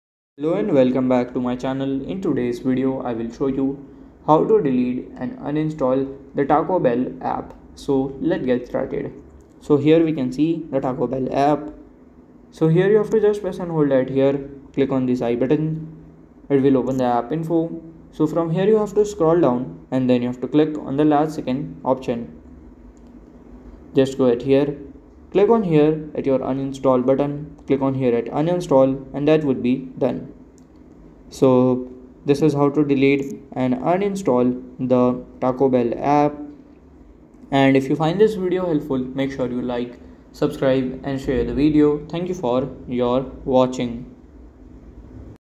✅How To Uninstall Taco Bell App And Cancel Account (Full Guide)
Ready to say goodbye to the Taco Bell app? This comprehensive guide provides step-by-step instructions for uninstalling the app and canceling your account. Whether you're exploring different fast food options or simplifying your digital tools, follow these detailed steps for a smooth process.

Key Learning Points:

Step-by-step guide to uninstalling the Taco Bell app
Instructions for canceling your Taco Bell account
Tips for managing your fast food subscriptions effectively